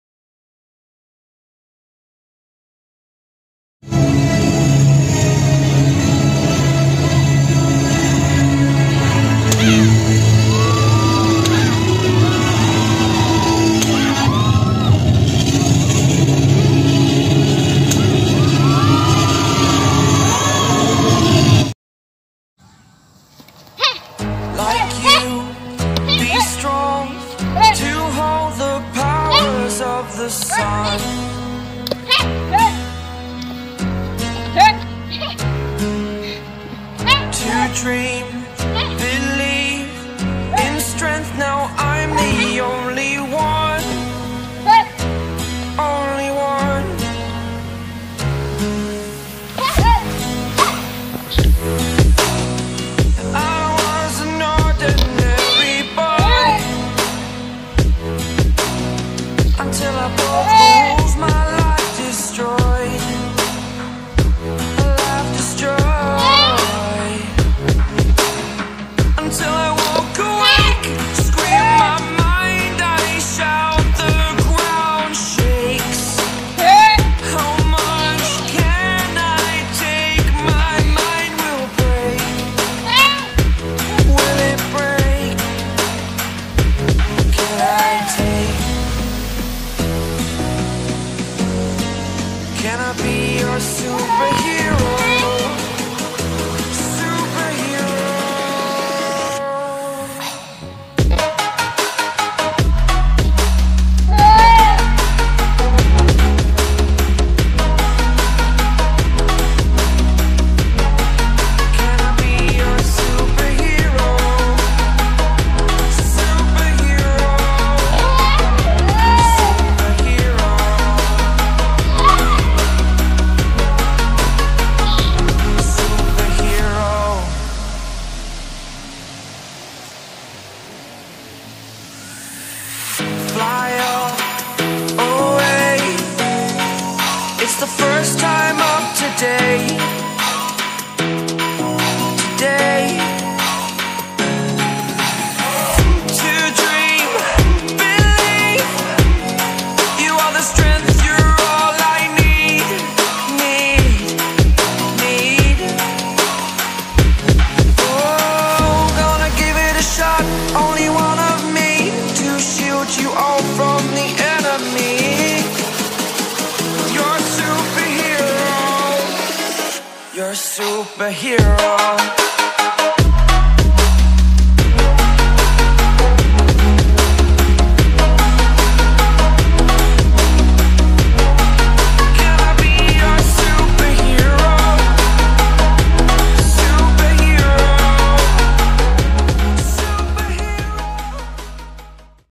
Taekwondo Girl vs two boys Real Fight
Hi everyone this video about taekwondo girl defense
,two boys
How effective is kung fu in a street fight?